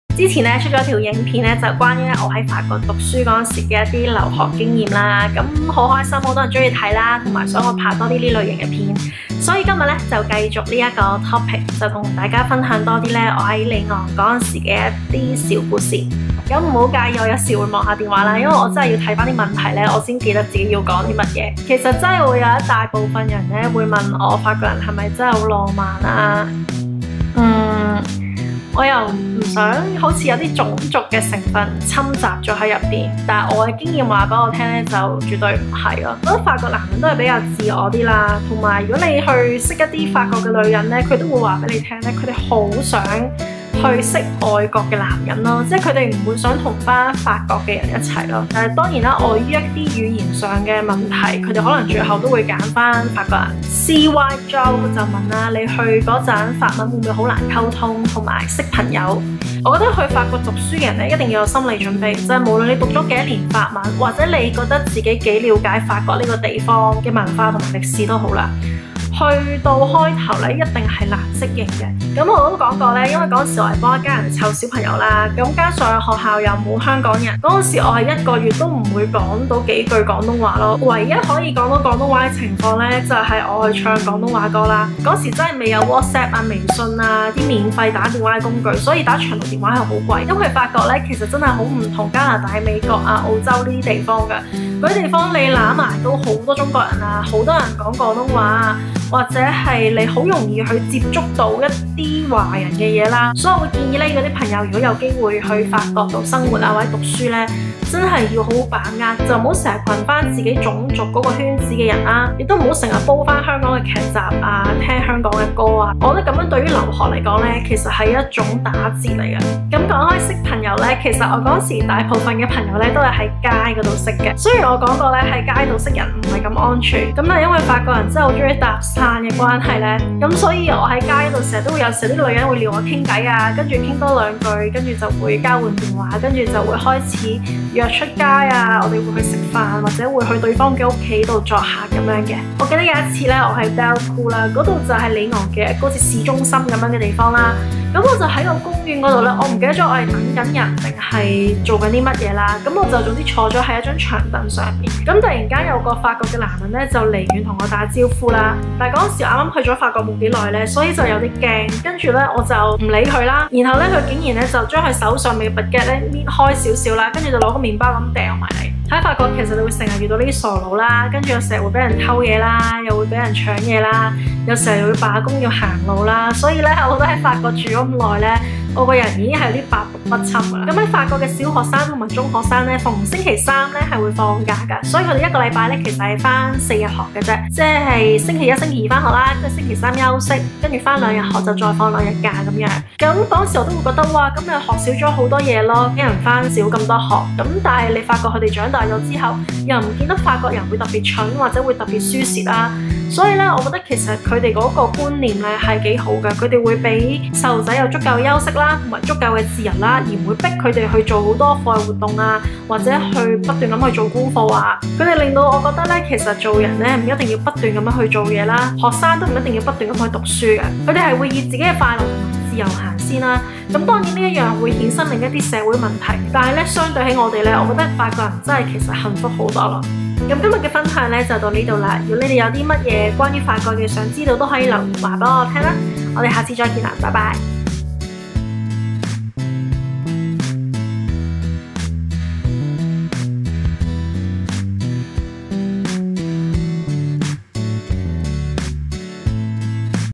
法國留學－我在里昂的日子 #3 法國的教育方式【留學雜談】｜陳怡 ChanYee
在法國留學，最叫我最震撼和深刻是他們對「自由和快樂」的重視，他們把人生的快樂放在第一位，因此我從未見過有父母會逼孩子去做不想做的事，亦因為如此，法國人對他人的包容度也較高（這跟華人社會恰恰相反）。

我在法國，最大得著不是學會法文、拿大學學位，而是學會這個地方的文化和價值觀，這些都是在自己國家永遠學不到的，其實這才是留學的真正的意義。